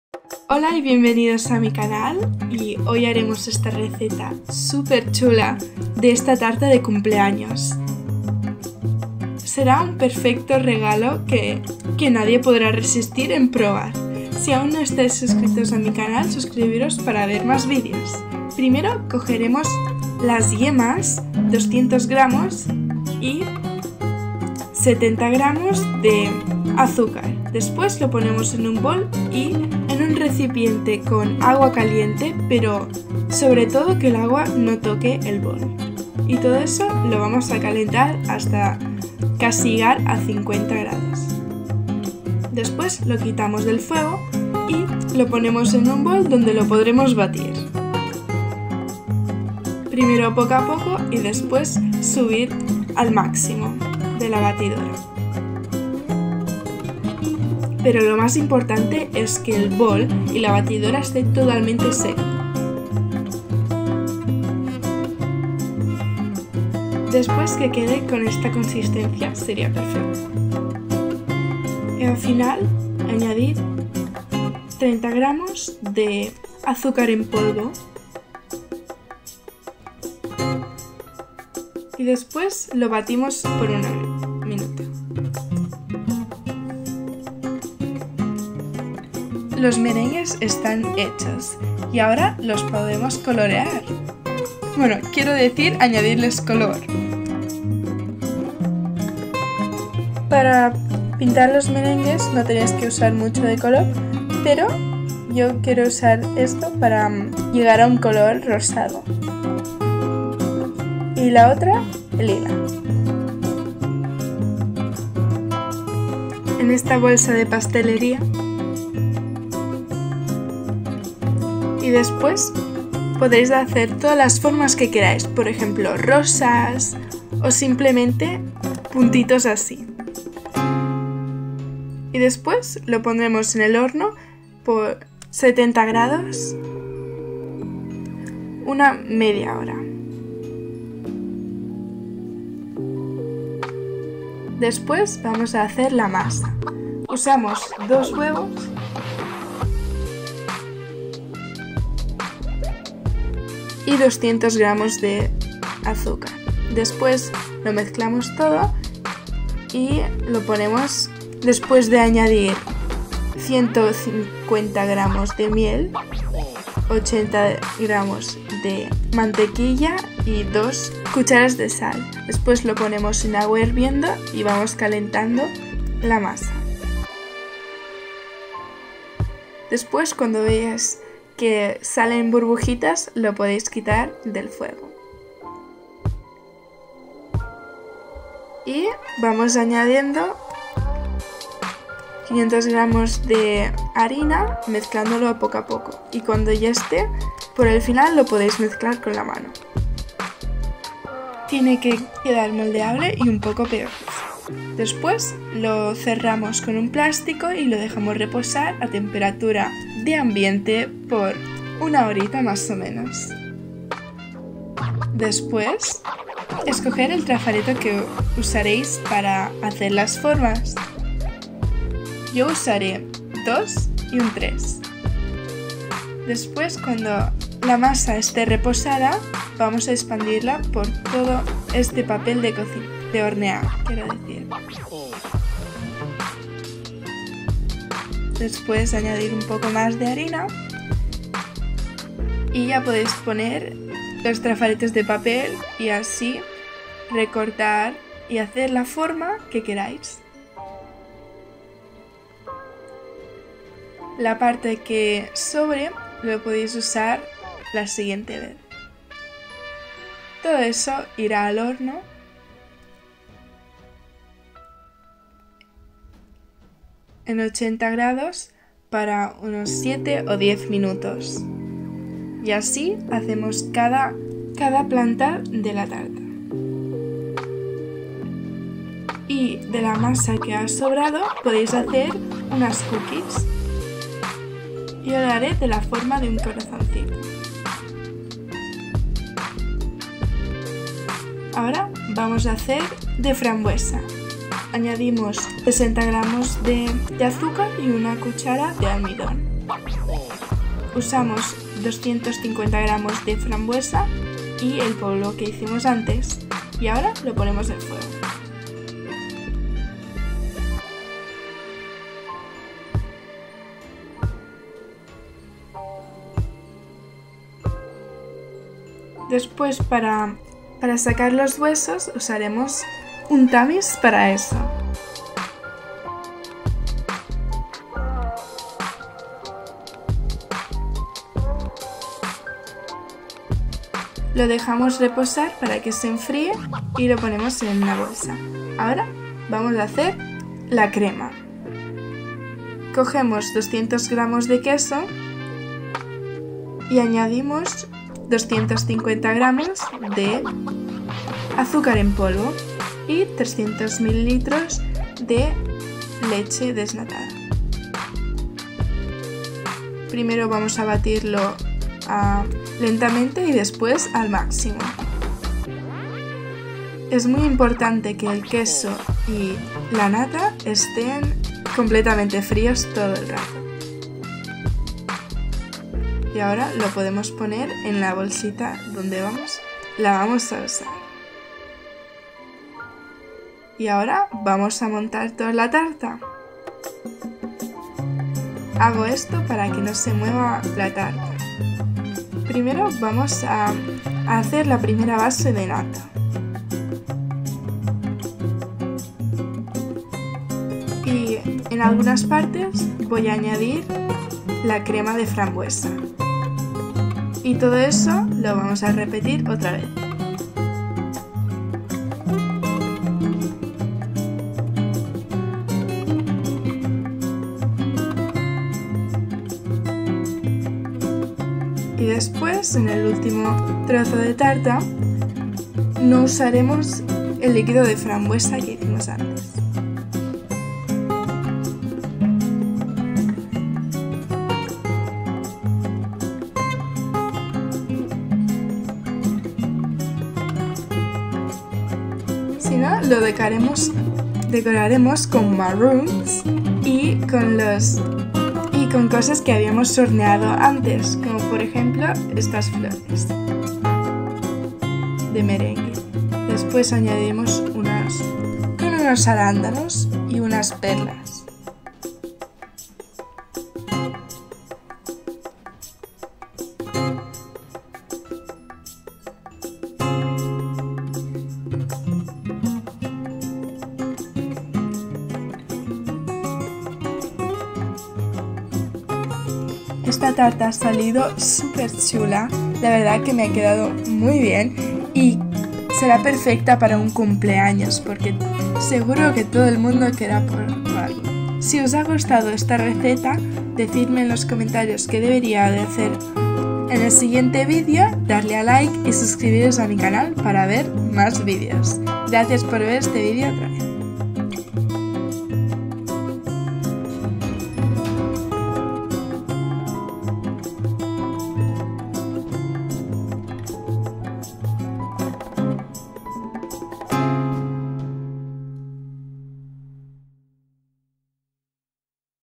🍰Pastel creativo NÚMERAL con flores de cumpleaños 🌸 Receta DIY fácil
La figura del pastel es una tendencia absoluta para un cumpleaños. Tal pastel es un gran regalo para cualquier persona de cumpleaños. Hoy prepararé un pastel para una niña en rosa y morado.
La base fue tomada por la receta de pastel favorita de todos, pastel de miel y una crema delicada.